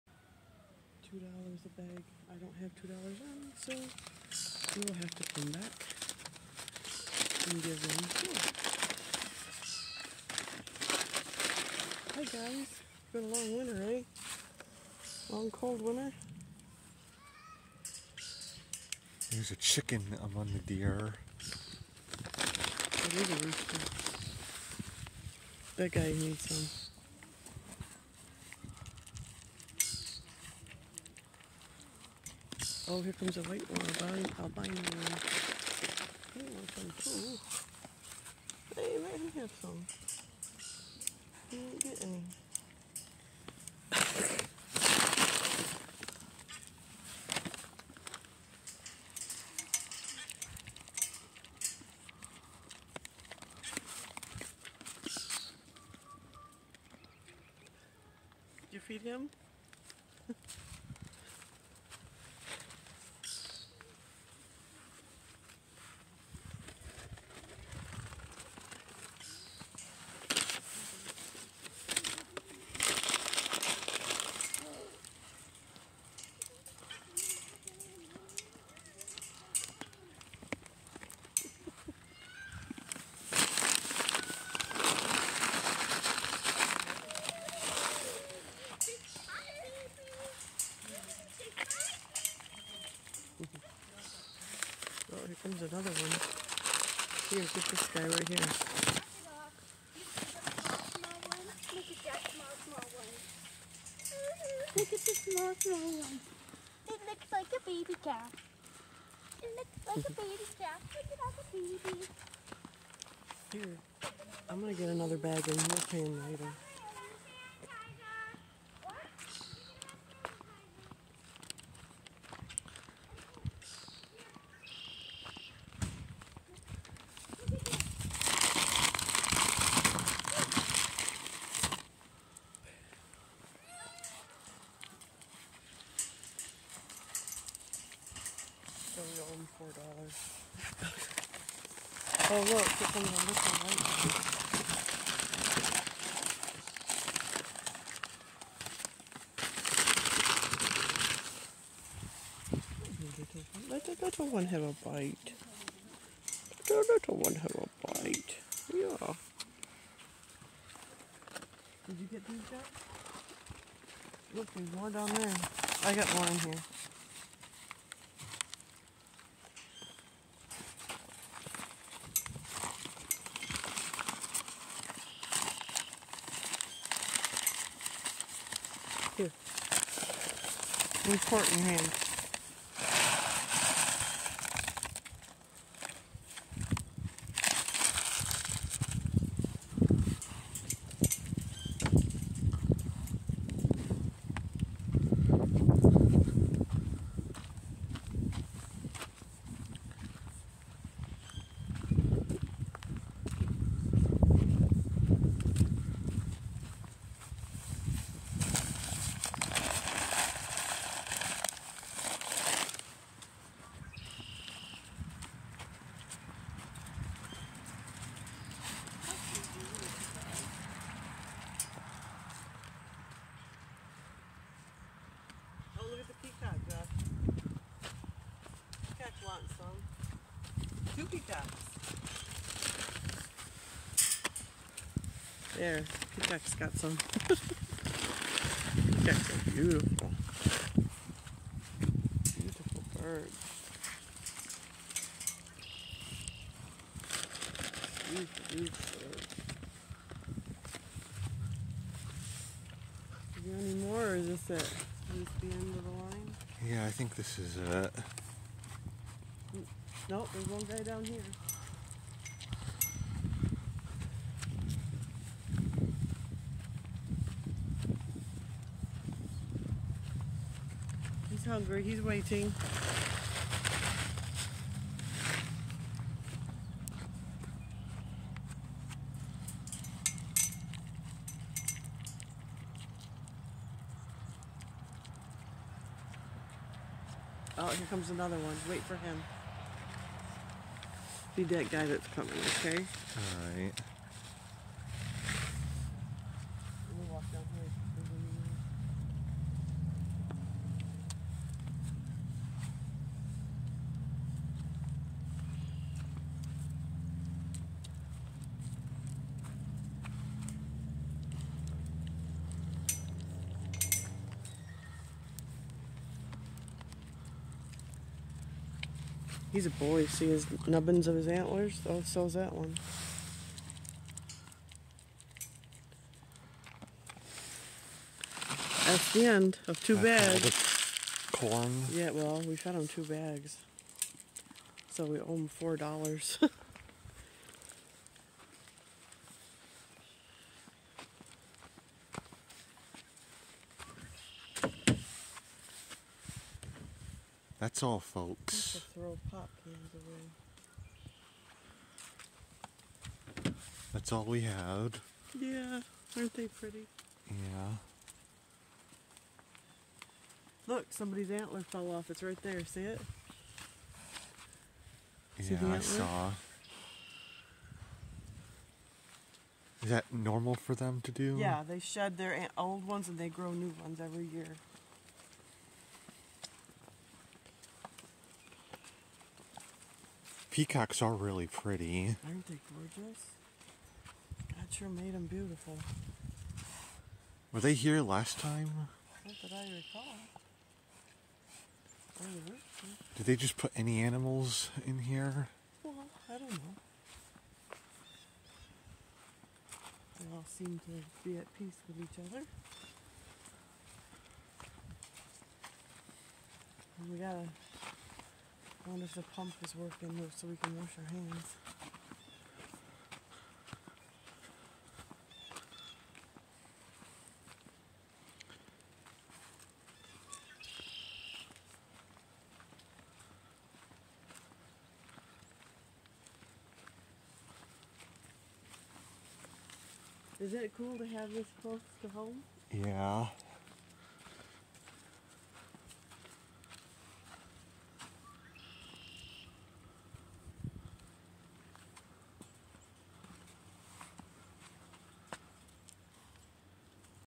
April Fools' Day 2024.
On this day of fools, me and my mom fed the deer here at Ogemaw Nature Park as well as 2 peacocks and a chicken.  This is the first time since the 900th video special back on July 9th, 2023, that we have come back here to feed these hungry creatures and there were quite a lot of deer here this time around more than the last time we were here.